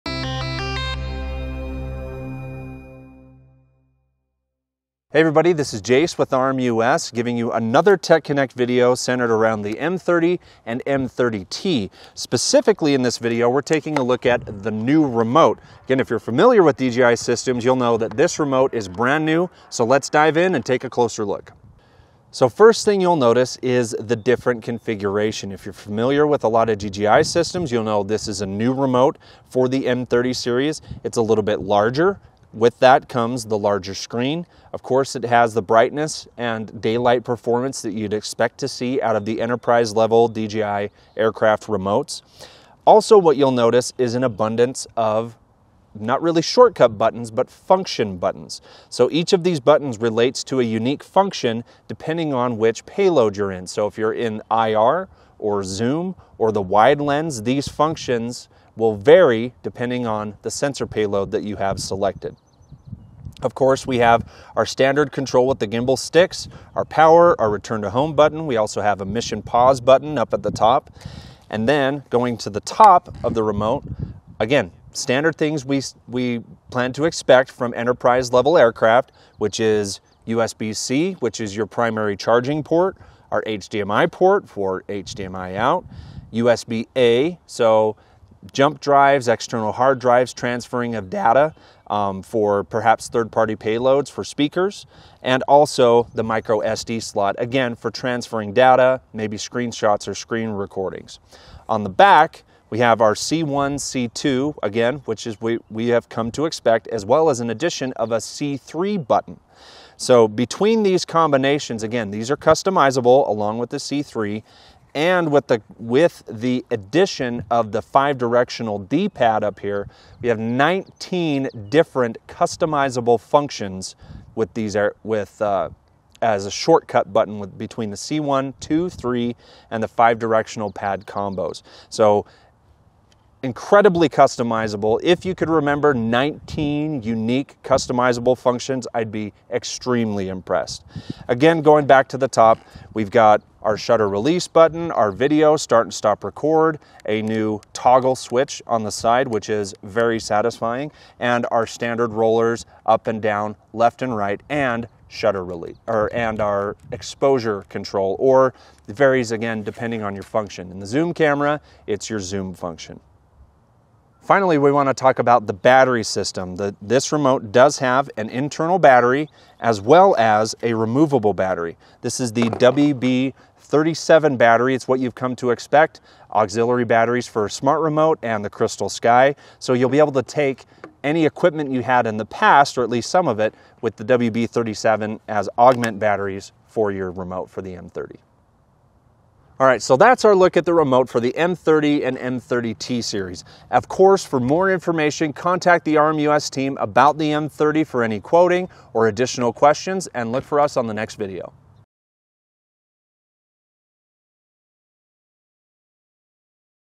RMUS TechConnectᵀᴹ | DJI M30 Remote
Jace Nelson takes a quick look at the DJI RC Plus remote controller featuring a 7-inch-wide high-definition screen. The IP54 certified controller sports six buttons in addition to the control sticks by the screen making piloting and switching between wide angle, zoom, infrared and FPV views easier.